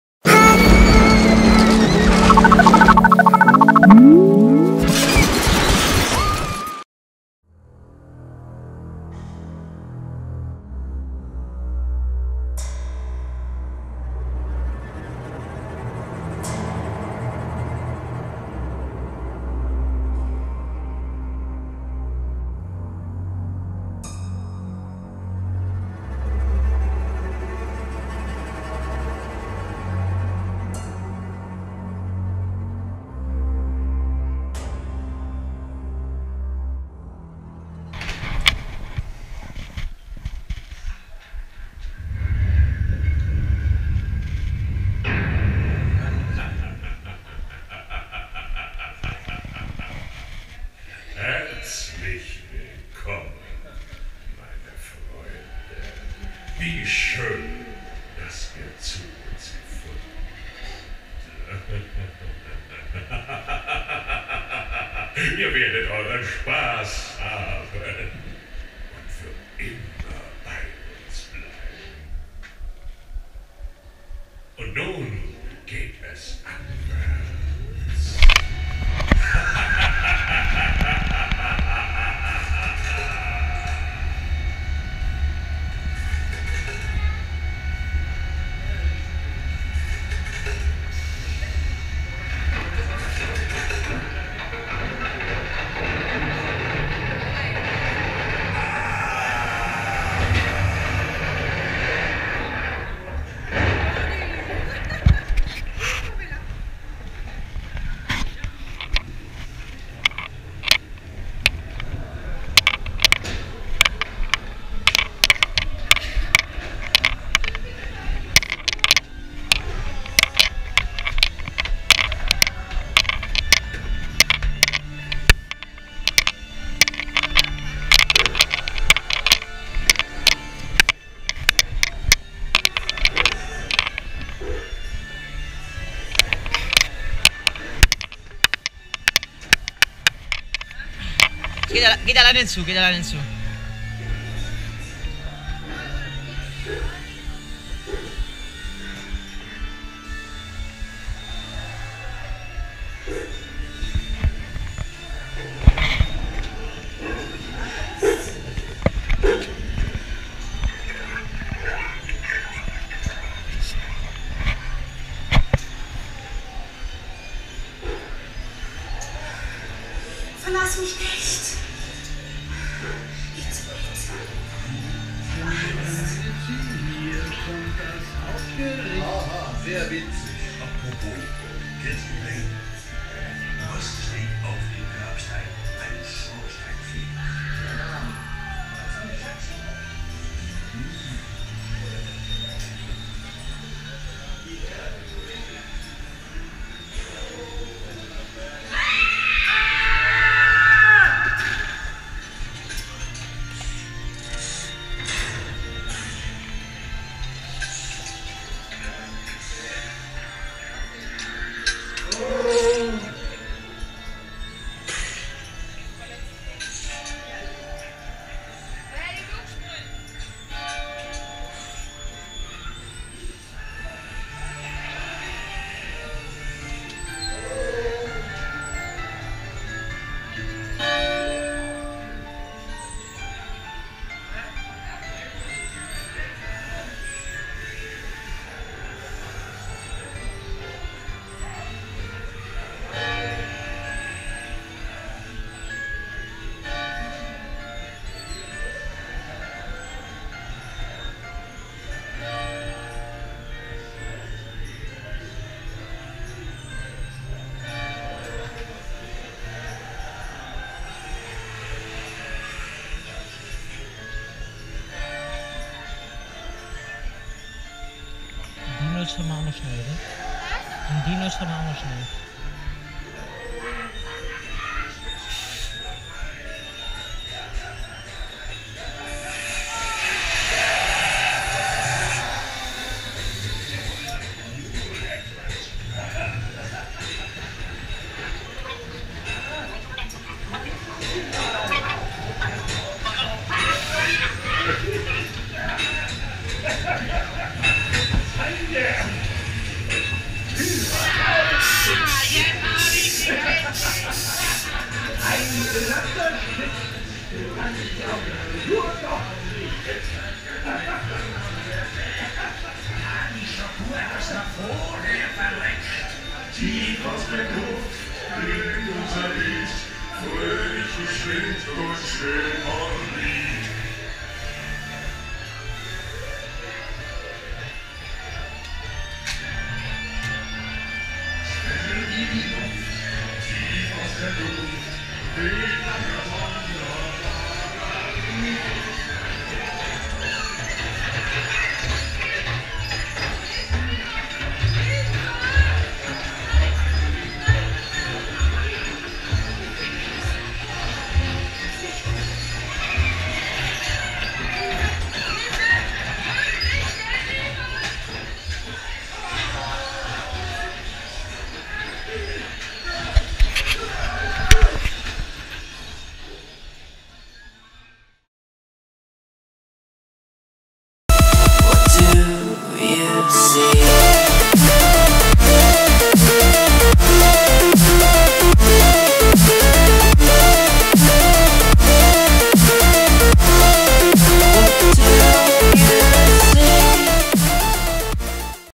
Geisterschloss - onride | Europapark Rust 2016
Ein Darkride mit Gruselgarantie! Wunderschön gestaltete Szenerien und eine lange fahrt warten im Geisterschloss!

Ab gehts in die schönsten Freizeitparks, Schwimmbäder und Anlagen sowie auf die schönsten Volksfeste: TheKirmesCrazy